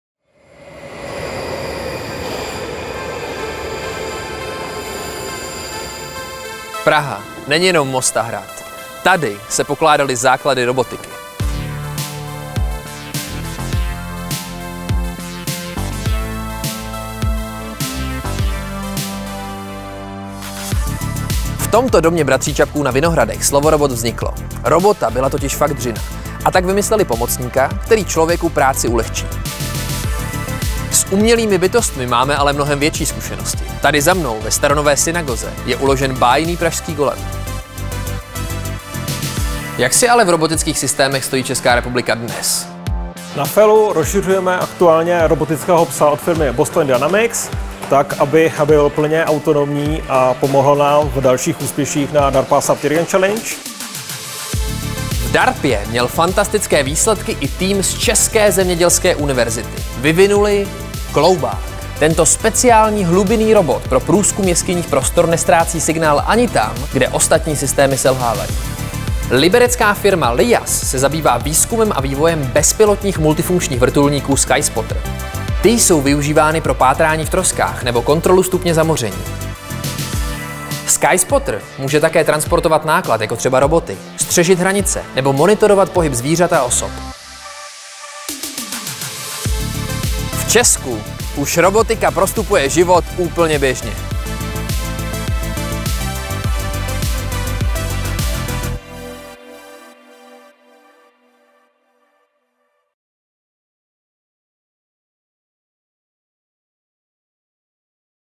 VimeCoMame 2 - 1. díl: Autonomie a robotika
Bude mít česká armáda svého Golema? Nebo se budou vojáci spíš spoléhat na robotického psa z ČVUT, hlubinného robota Kloubáka z České zemědělské univerzity či na bezpilotní vrtulník, jaký vyvinula třeba česká firma LIAZ?
O rozvoji robotiky v českých zemích hovoří v 1. díle nové série VimeCoMame náměstek ministryně obrany Tomáš Kopečný.